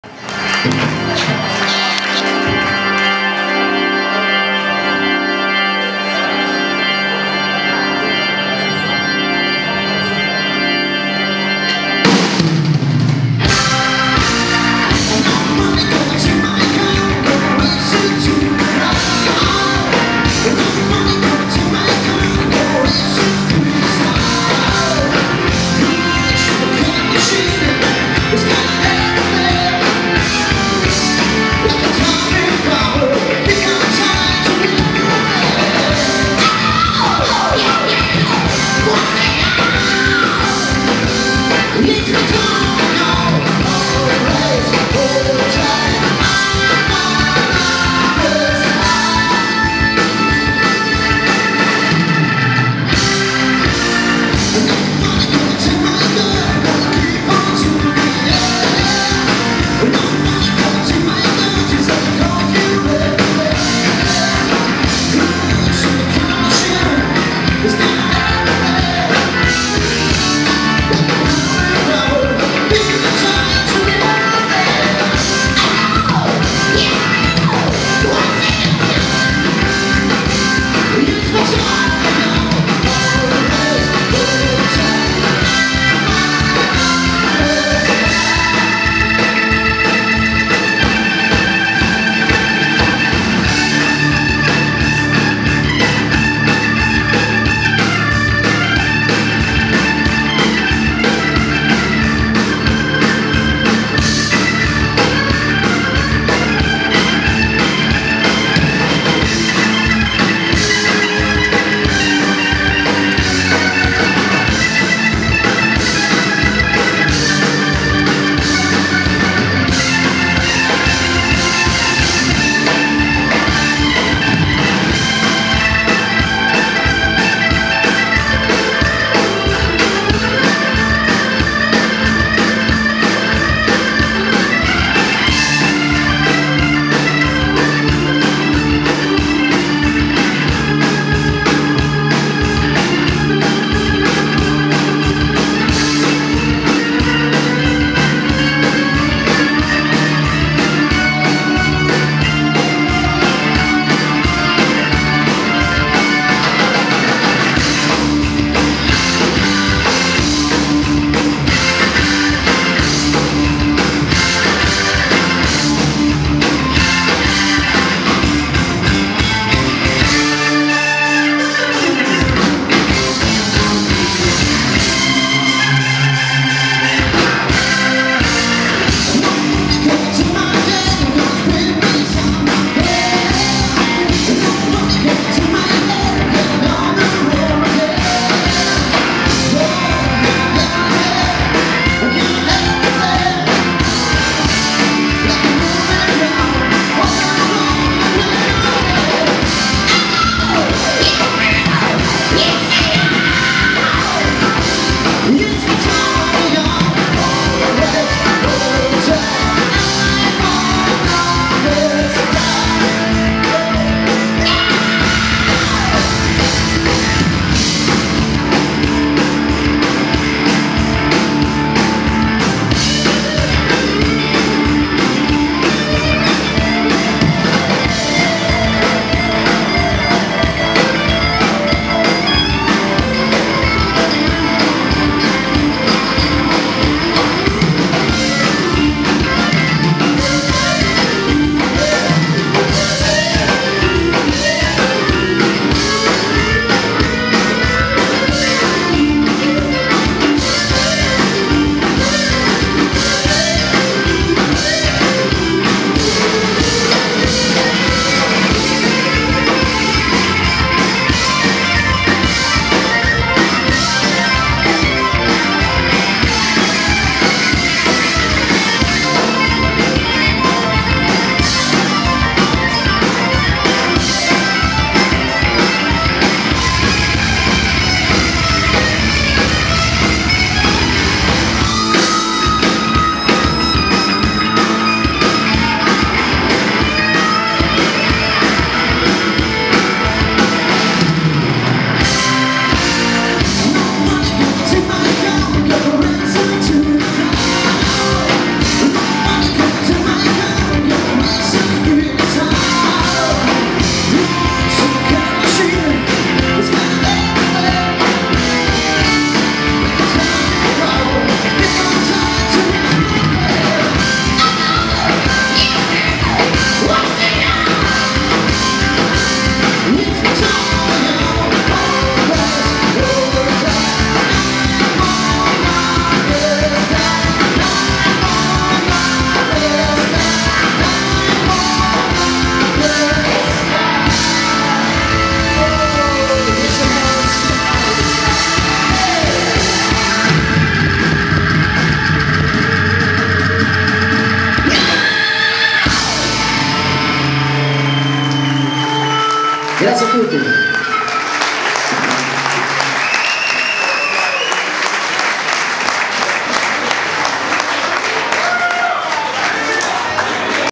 SILVER MIRROR ! LIVE AL DON CHISCIOTTE !
Non si poteva iniziare meglio il 2013...gran bella musica con i mitici Silver Mirror e tantissima bella gente...grazie di cuore a tutti !